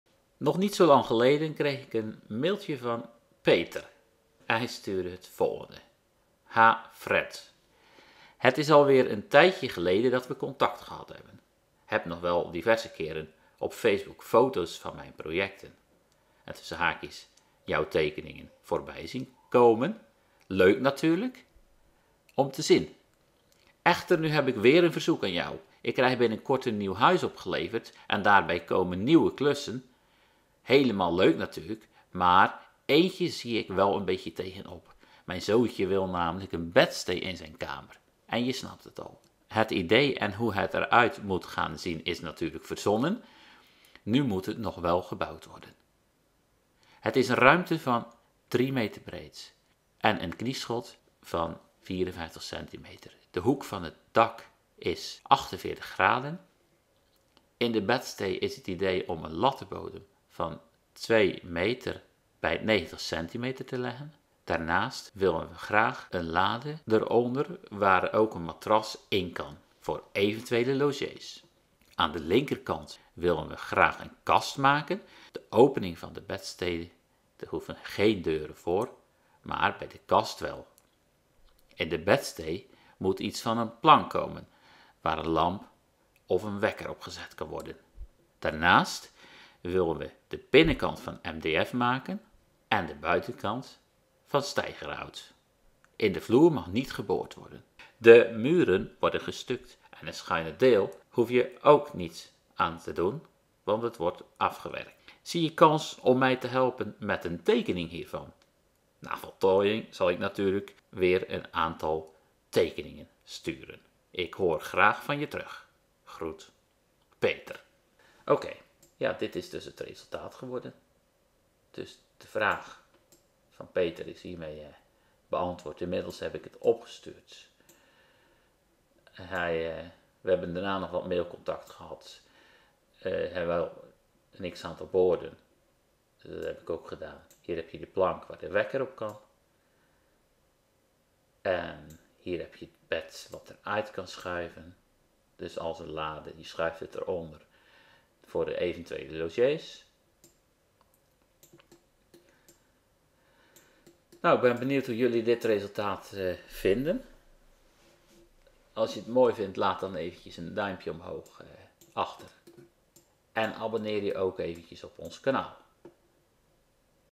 Bedstee onder schuine dak met kast | bedstee bouwtekening
Bedstee onder schuine dak met kast staat in het premium gedeelte van de

Van Peter kreeg ik de volgende vraag:

Ha Fred,

Het is al weer een tijd geleden dat we contact hebben gehad. Heb nog wel diverse keren op facebook foto's van mijn projecten (en jouw tekeningen) voorbij zien komen!! Leuk om te zien.

Echter nu heb ik weer een verzoek aan jou. Ik krijg binnenkort een nieuw huis opgeleverd en daarbij komen nieuw klussen :-). Helemaal leuk natuurlijk, maar eentje zie ik wel een beetje tegenop. Mijn zoontje wil namelijk een bedstede op zijn kamer. Je snapt het al. Het idee en hoe het er uit moet gaan zien is natuurlijk verzonnen. Nu moet het nog gebouwd worden :-).

Het is een ruimte die 3 meter breed is en een knieschot heeft van 54 cm. De hoek van het dak ik 48 graden (zie foto). In de bedstede is het idee om een lattenbodem van 200*90 cm te leggen. Daarnaast willen we er graag een lade onder waar ook een matras in kan (voor evt logees).

Aan de linkerkant willen we dan graag een kast maken. De opening van de bedstede hoeven geen deuren voor, maar bij de kast wel.

In de bedstede moet iets van een plank komen waar een lamp / wekker gezet kan worden.

Daarnaast willen we de binnenkant van MDF maken (of zou je dat met iets anders beter kunnen doen?) en de buitenkant (aanzicht) met steigerhout.

In de vloer mag / kan niet geboord worden.

De muren worden gestuct en het schuine deel wordt ook netjes afgewerkt. (lees: hoeft dus niet dicht gemaakt te worden.

Zie jij kans om mij te helpen met een tekening hiervan? (Na voltooiing zal ik je natuurlijk weer vaneen aantal foto's voorzien.

Hoor het graag van je terug.

Groet,

Peter